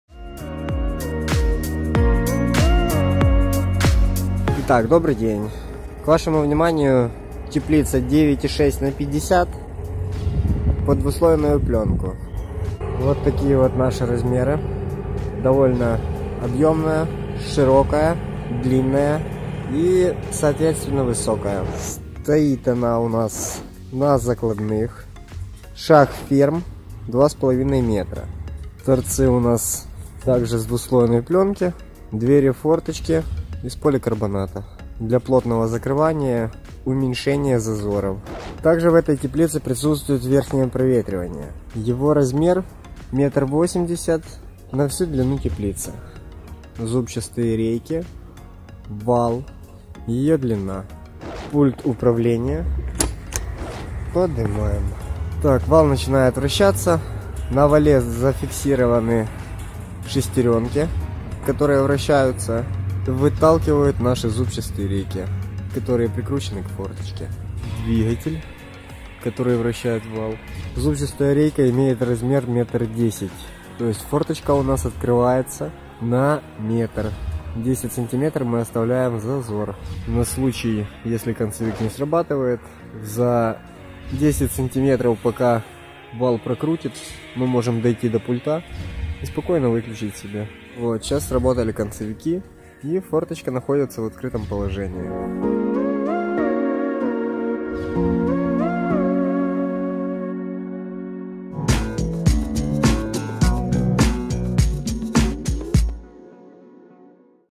Верхнее проветривание ФЕРМЕРСКИХ ТЕПЛИЦ
Более подробно про варианты вентиляции теплицы, можно посмотреть на нашем сайте также, можете звонить по номерам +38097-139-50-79 или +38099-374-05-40 viber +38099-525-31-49

Проветривание фермерских теплиц - задача не из легких, а особенно, если теплица накрывается пленкой. Нами было принято решение: установить автоматическую форточку в коньке по всей длине теплицы. Данное расположение, позаимствовано у теплицы Митлайдера, которая как известно, славится эффективным проветриванием, за счет выброса тёплого воздуха из под купола теплицы. Поэтому, за дефицит свежего воздуха в данной теплицы можно не переживать.

Полезные ссылки:
Вентиляция теплиц